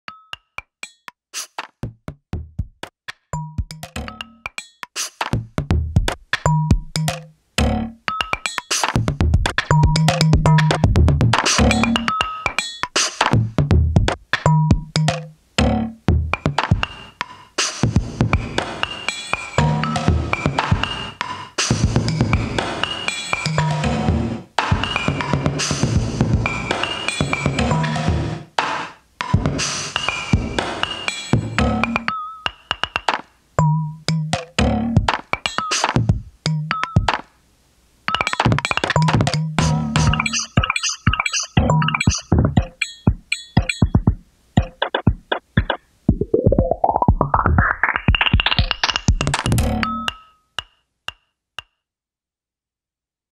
53 Seconds of Percussion
18 percussion sounds, 5 Goodhertz plugins, 53 seconds